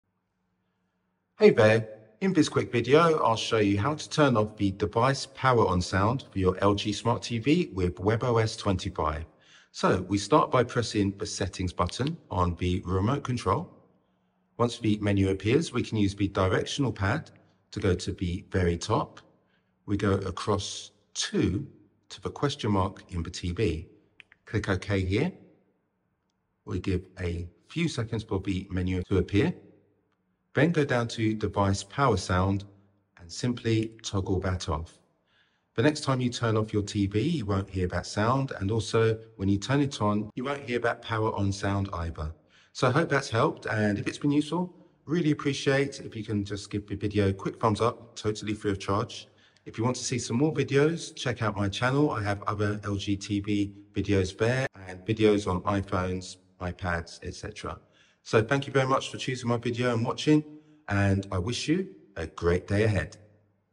LG TV - Turn Off Startup Sound - 4K - Step by step
Here's how to turn off the startup sound and the shutdown sound for your LG Smart TV with WebOS 25 - step by step instructions for you.